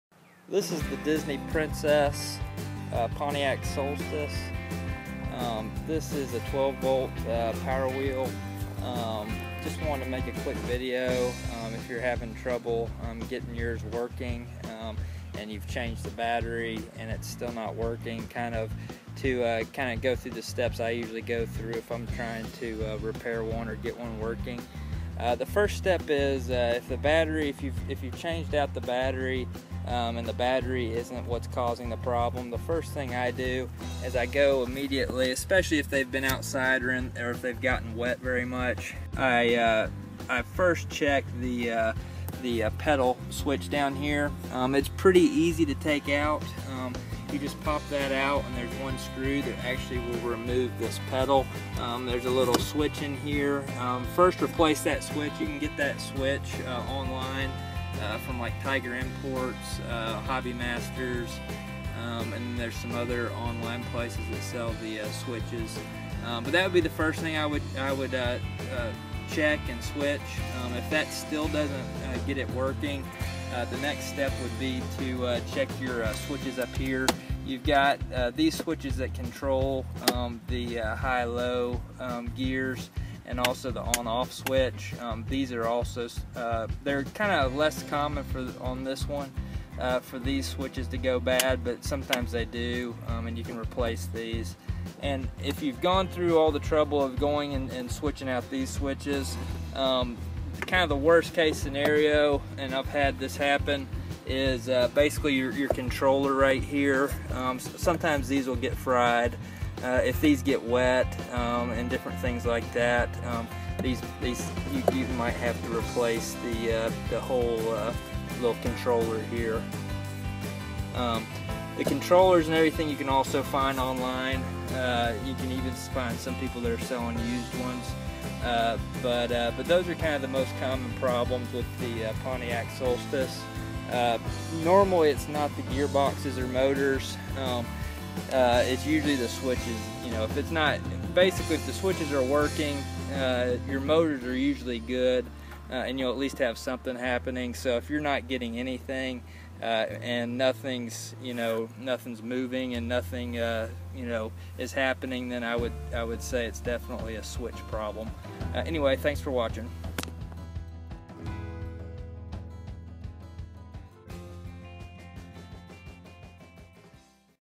How To Fix MotionTrendz Disney Princess Pontiac Solstice Power Wheels - Troubleshooting Repairs
How To Fix MotionTrendz Disney Princess Pontiac Solstice Power Wheels - Troubleshooting Repairs.  Owners Manual - Common problems and repairs on the Princess Solstice Power Wheel.